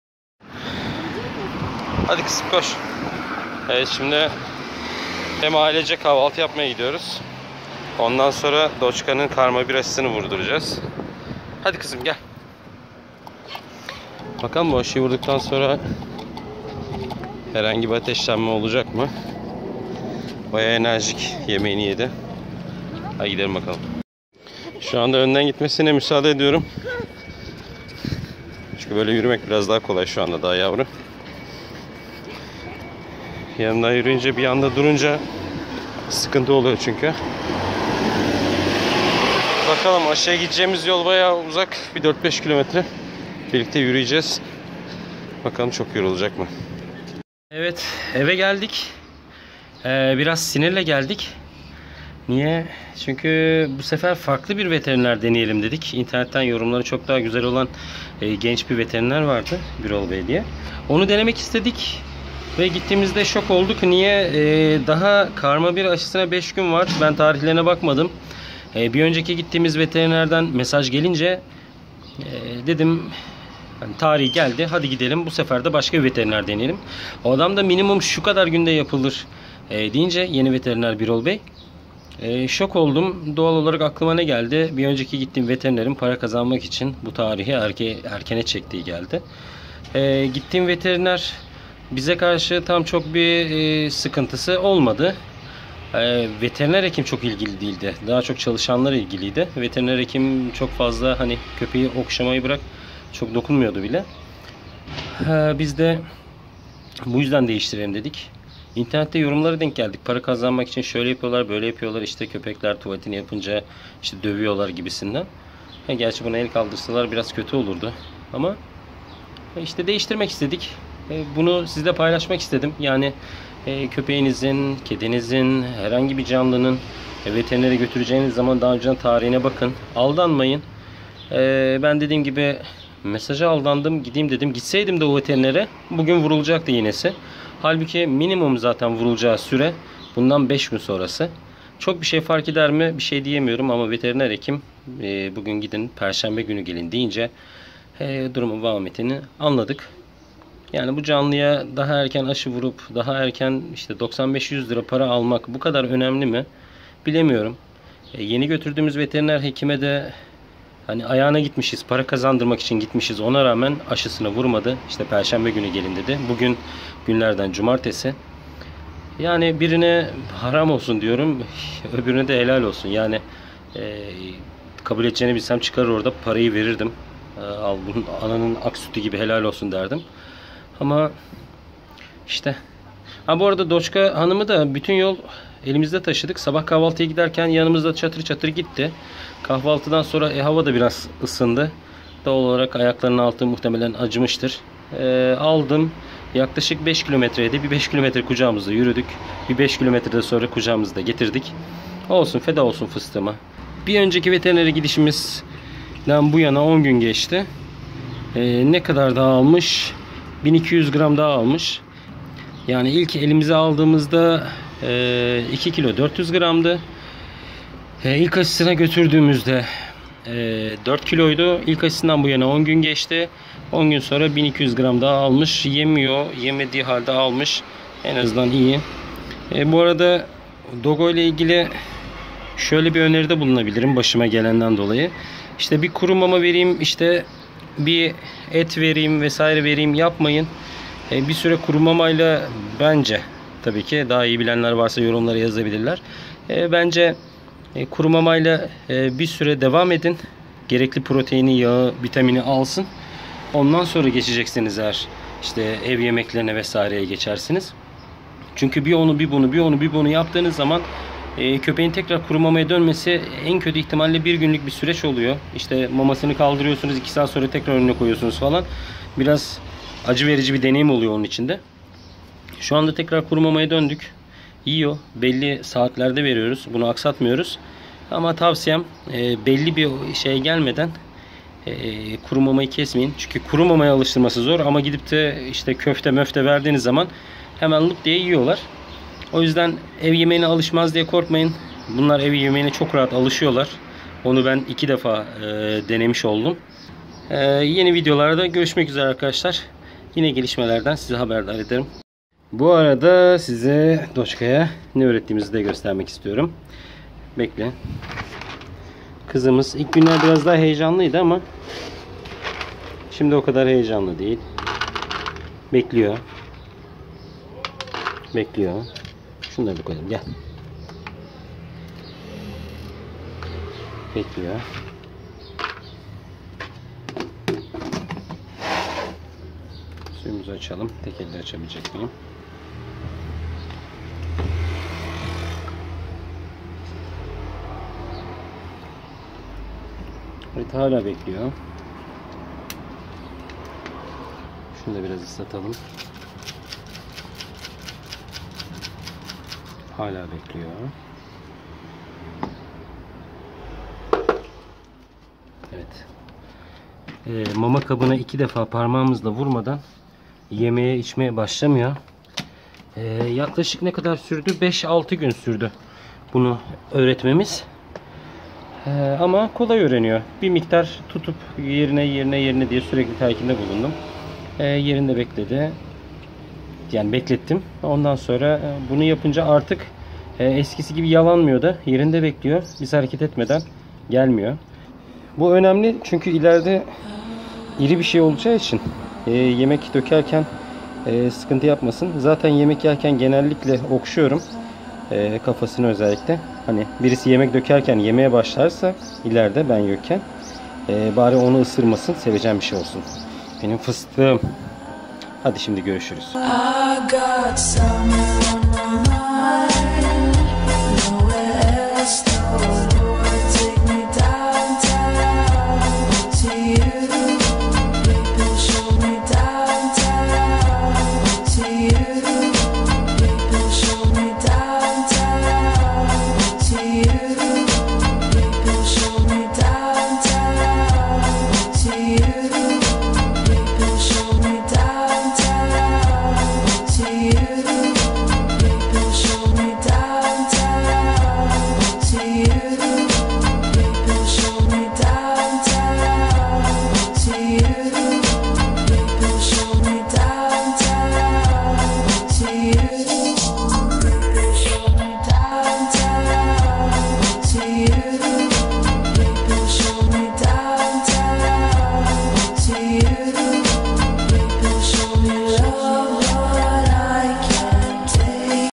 DOGO ARGENTINO - VETERINER AŞI TUZAĞI - DOCHKA (DOÇKA) - KARMA 1 AŞISI
Herkese Selam,
Dochka ile ilgili ufak bir kaç görüntüyü paylaşmak istedik.
İkinci aşısını (Karma 1) vurdurmak için farklı bir veterinere gidince, daha önce gittiğimiz veterinerin aşıyı erken vurmaktan çekinmediğini düşündük.
iki seferdir gittiğimiz veterinerde sorun yaşamadık ama hekimden yavruya ilgi de görmüyorduk. İnternette de hakkında kötü yorumlar görünce farklı veteriner arayışına girdik.

Deneyimlediğim şey: Veterinerden mesaj gelse de aşı tarihi hakkında kontrolünüzü yapmadan aşıyı vurdurmayın.

Kilo değişimi:

19 Haziran 2019 = 2400 Gr.
03 Temmuz 2019 =  4000 Gr.
13 Temmuz 2019 =  5200 Gr.

Bu canlardan birine sahipseniz ve deneyimlerinizi bizimle paylaşırsanız çok memnun oluruz.

Umarım izlerken sizler de keyif alırsınız.